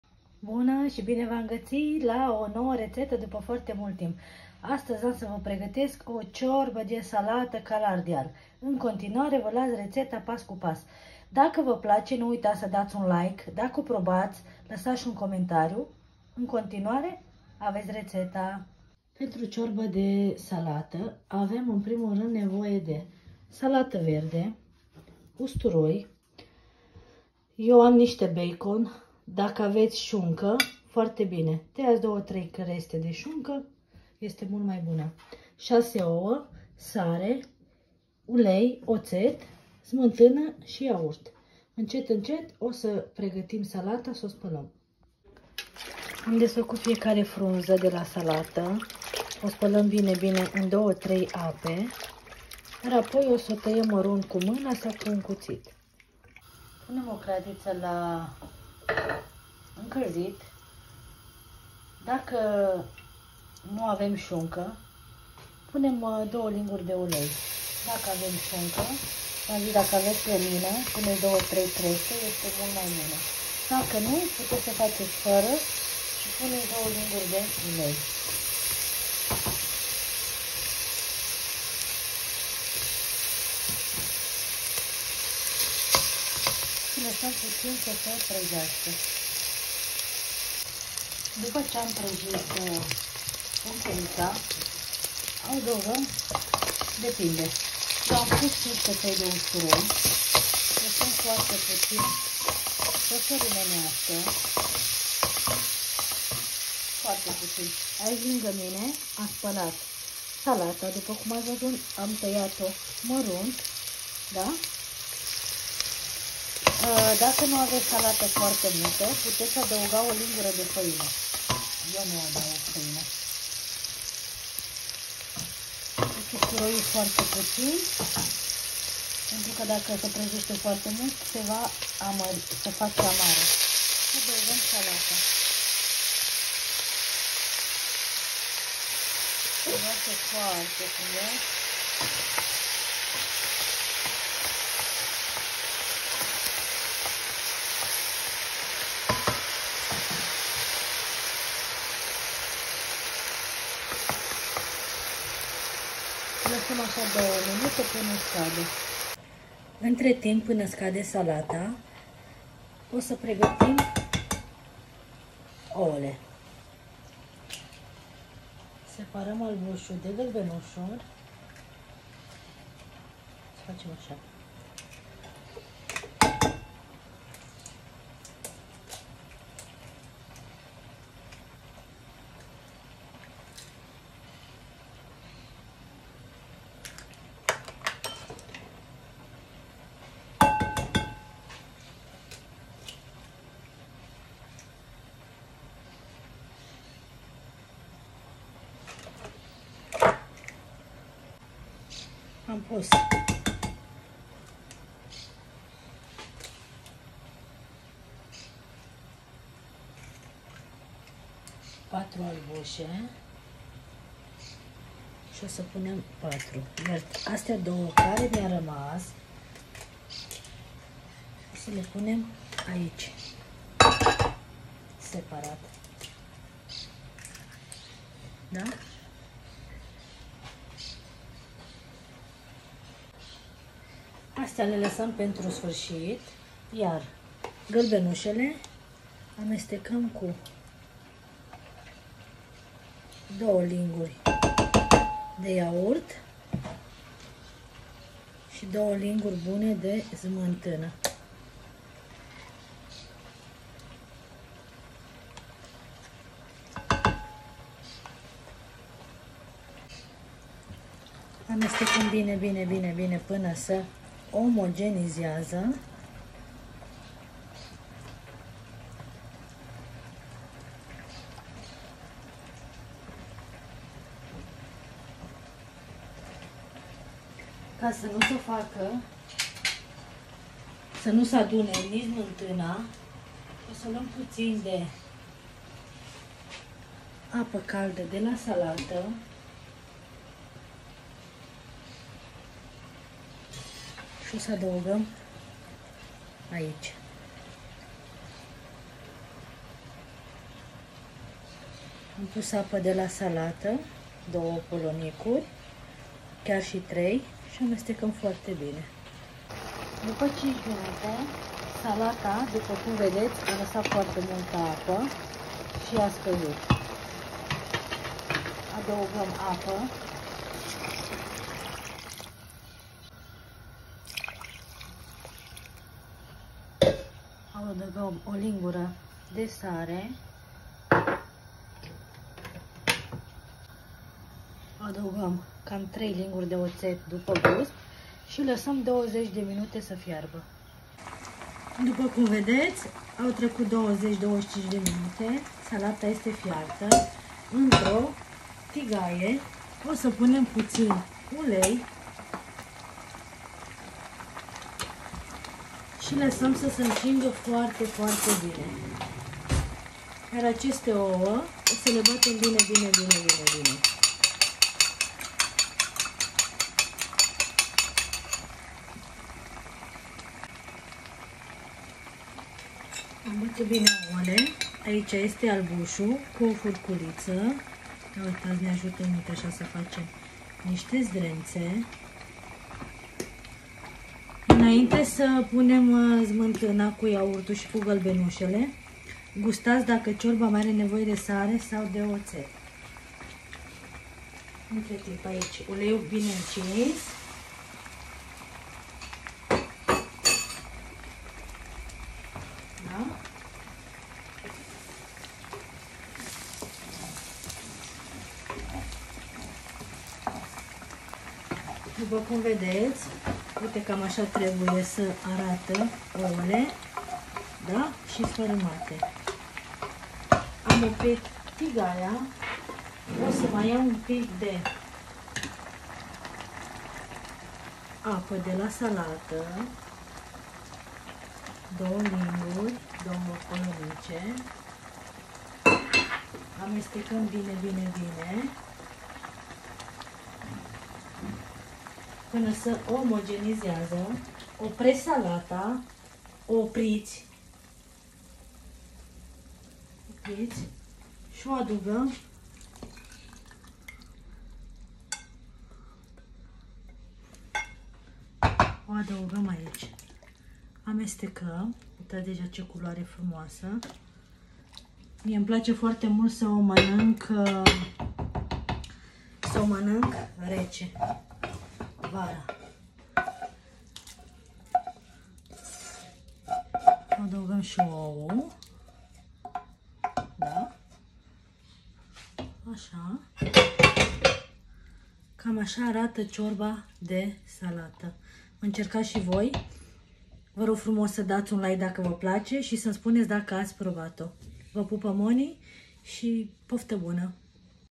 Ciorba de Salata (Ardeleneasca )
Salata 2,3 căpățâni
usturoi 4,5 căței
oua 6
Șunca (slănina )3,4creste (bucăți )
iaurt 2 linguri mari
# smântâna 2,3 linguri mari
sare după gust
otet după gust
# ulei 1,2 linguri
Vizionare placuta ❤️